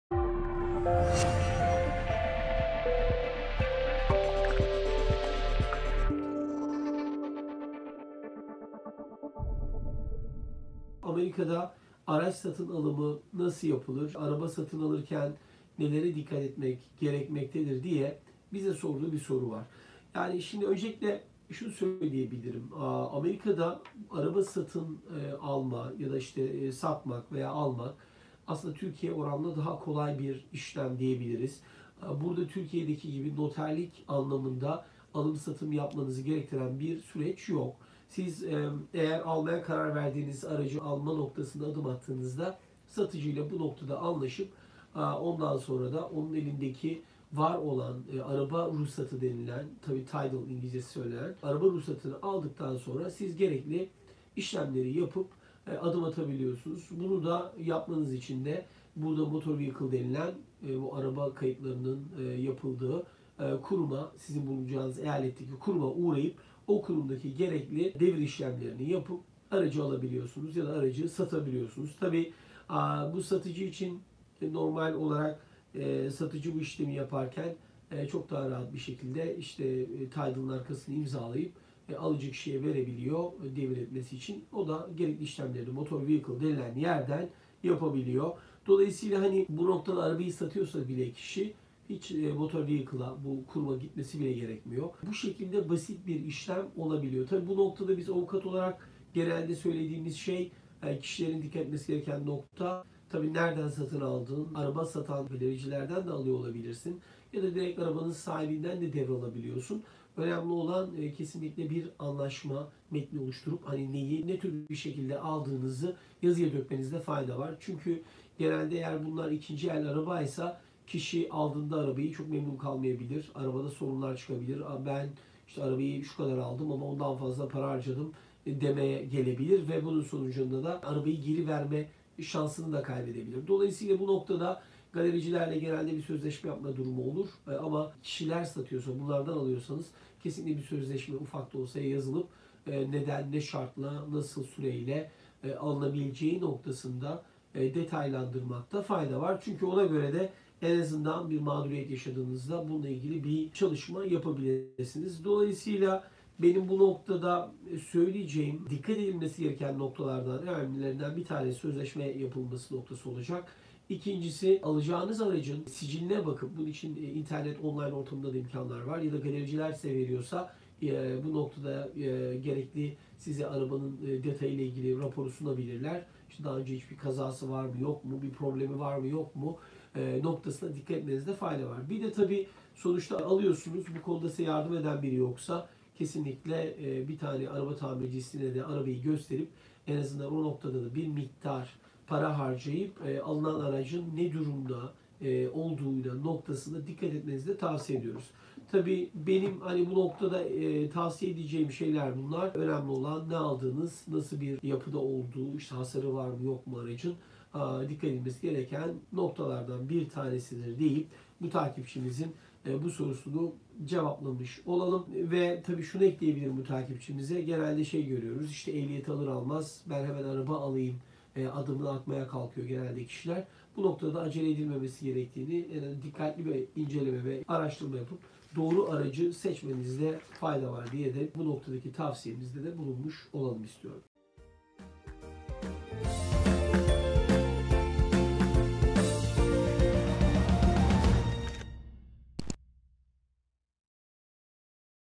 AMERİKA'DA ARAÇ ALIM-SATIMI NASIL YAPILIR ?
Soru: Amerika'da araç satın alımı nasıl yapılır, araba satın alırken neye dikkat etmek gerekmektedir ?